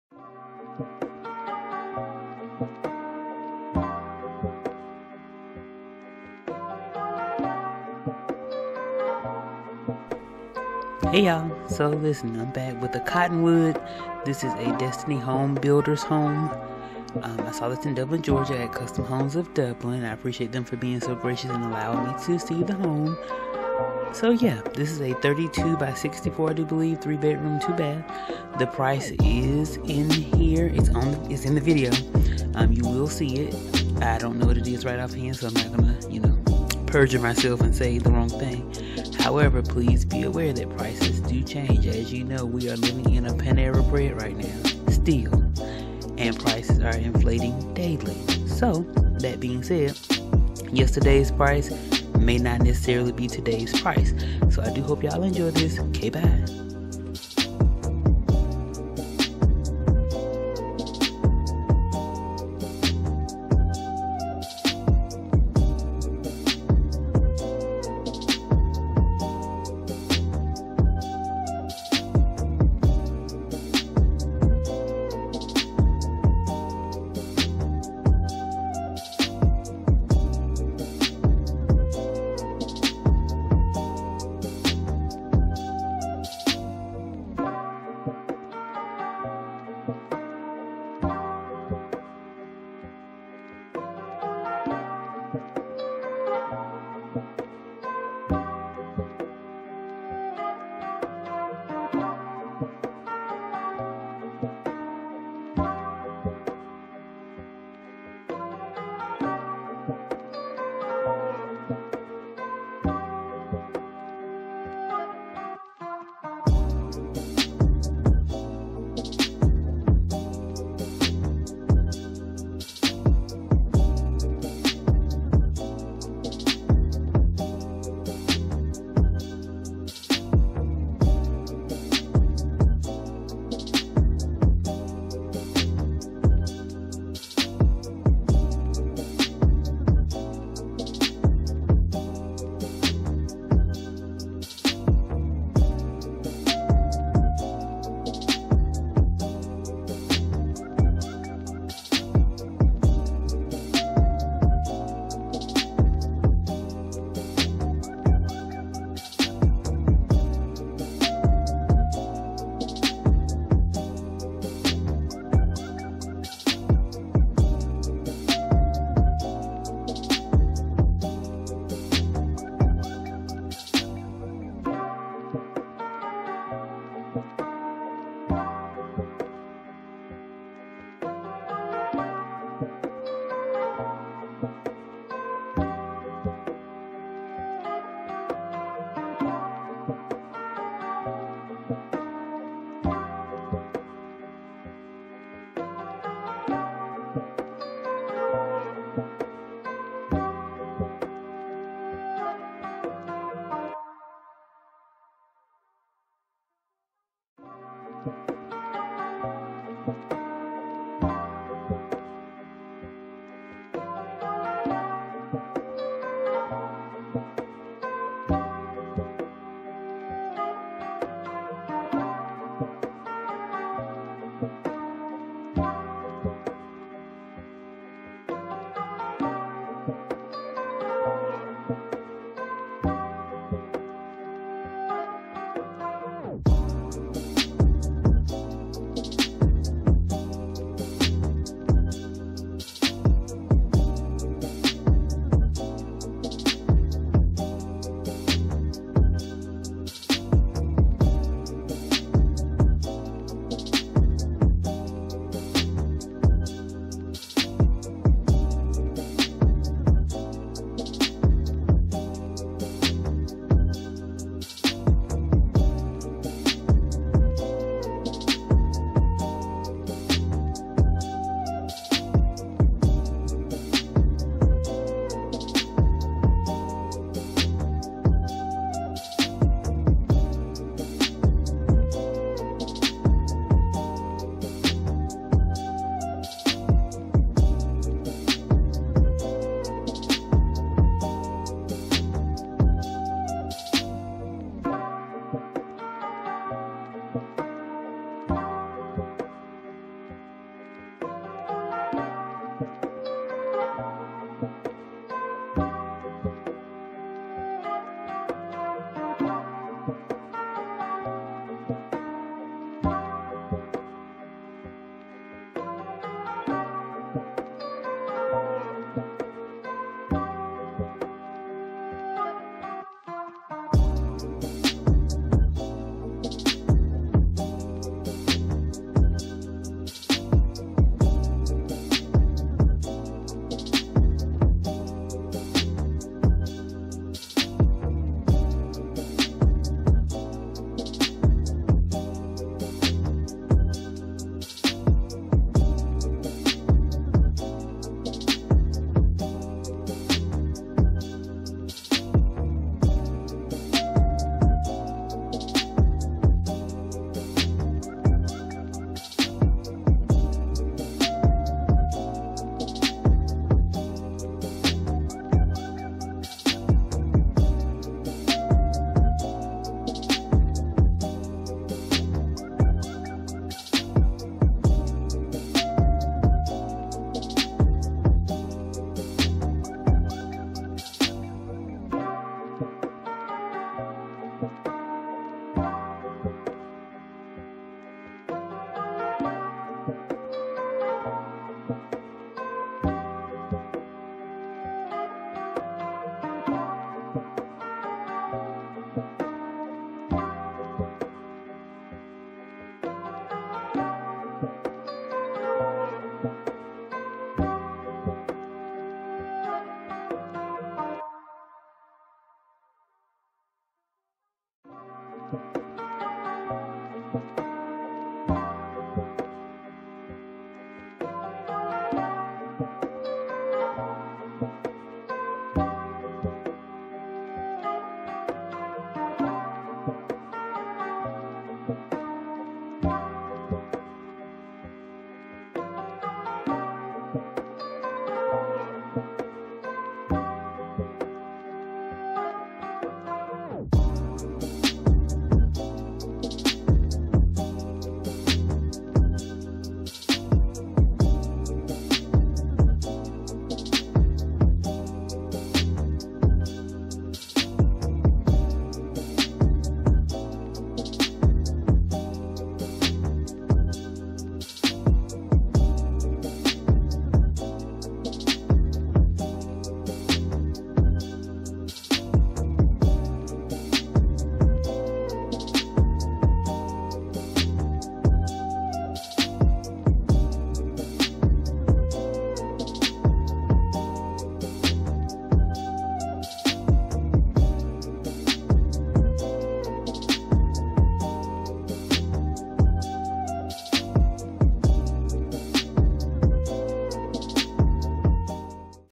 THE COTTONWOOD | DESTINY HOMEBUILDERS | Mobile Home Review
**OPEN ME***
Hey Yall Hey!

Here is another manufactured home walk-through!

Below you will find:
The dealers website where I viewed the home, interactive walk-throughs(if available).
*please note* Often different home manufacturers have the same floor plan by another name.)
I will also include any pricing and other details provided.

This home features:
BEDS: 3     BATHS: 2     SQ FT: 1780 W X L: 32′ 0″ x 64′ 0″

Prices vary so please see your local dealer for pricing.